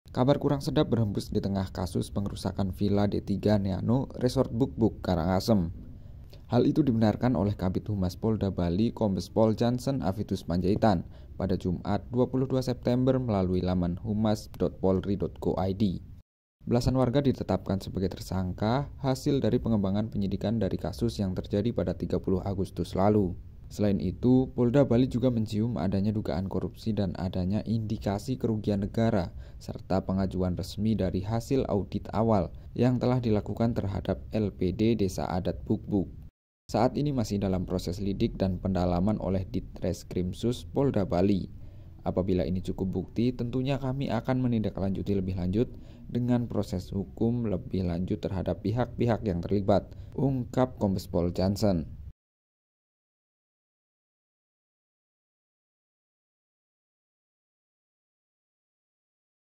"Aroma" Korupsi Tercium Polisi di Kasus Villa Bugbug?
Kabar kurang sedap berhembus ditengah kasus pengerusakan Villa Detiga Neano Resort Bugbug Karangasem.

Hal itu dibenarkan oleh Kabid Humas Kombes Pol Jansen Avitus Panjaitan SIK, MH, pada jumat 22 September melalui laman humas.polri.go.id

Belasan warga ditetapkan sebagai tersangka hasil dari pengembangan penyidikan dari kasus yang terjadi pada 30 agustus 2023 itu.

"Sampai saat ini sudah menetapkan 16 tersangka karena terbukti melanggar Pasal 187 KUHP, dan atau pasal 170 KUHP, dan atau pasal 406 KUHP, dan pasal 167 KUHP, dan atau pasal 55 KUHP," ujarnya dilansir dari humas.polri.go.id, Rabu, 27 September.

Selain itu Polda Bali kembali mencium adanya dugaan korupsi dan adanya indikasi kerugian negara serta pengajuan resmi dari hasil audit awal yang telah dilakukan terhadap LPD Desa adat Bugbug.

"Saat ini masih dalam proses lidik dan pendalaman oleh Ditreskrimsus Polda Bali, apabila ini cukup bukti tentunya kami akan menindaklanjuti lebih lanjut dengan proses hukum lebih lanjut terhadap pihak-pihak yang terlibat" ungkap Kabid Humas." Pungkasnya.

Video lengkapnya di YouTube Gema Bali dengan judul " 'Aroma' Korupsi Tercium Polisi di Kasus Villa Bugbug?"

GEMA BALI - inspirasi dari BALI